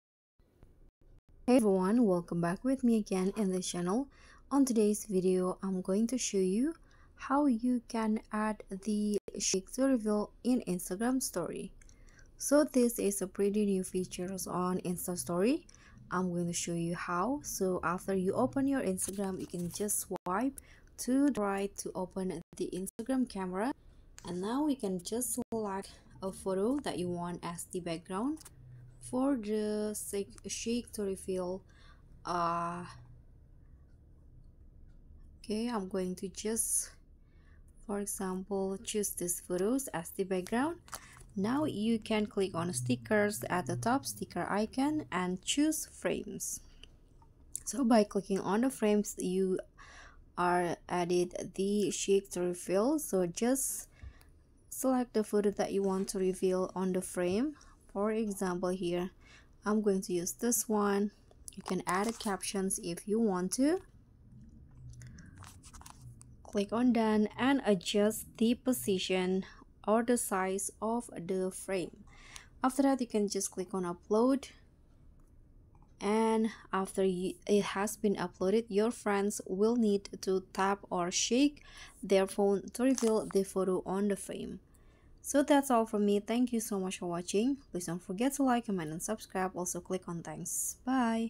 ❤ MASTERCLASS: How To Add Shake To Reveal in Instagram Story | Use Shake It Filter
This is how you can use the Shake to Reveal on Instagram story